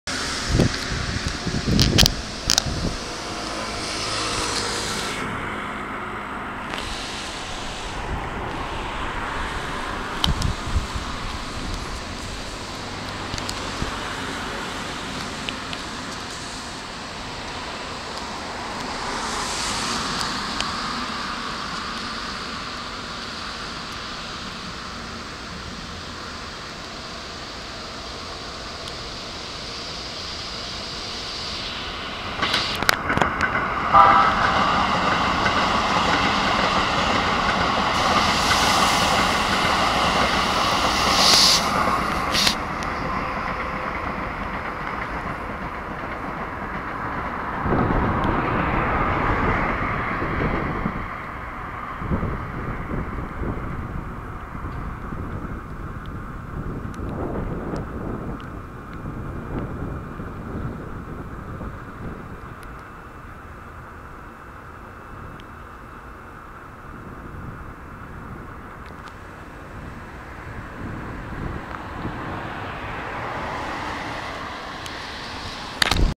京急線79H京急ブルースカイトレイン回送600形606-1〜606-8編成が金沢文庫検車区に停車中！エアポート急行逗子・葉山行き1000形473〜476+453〜456編成が金沢八景駅に到着！
京急線金沢文庫駅〜金沢八景駅間の金沢文庫検車区付近の歩道から撮影。

サムネイルの写真の撮影者も猫ぷに。(令和2年7月28日火曜日14時58分撮影)

令和2年7月28日火曜日14時57分撮影。

金沢文庫検車区に停車中の、8両編成79H京急ブルースカイトレイン回送600形606-1〜606-8編成。

金沢文庫駅を発車して走行中の、下り8(4+4)両編成エアポート急行逗子・葉山行き1000形473〜476+453〜456編成ステンレス車両が、金沢文庫第2踏切を通過して金沢八景駅下り2番線ホームに到着。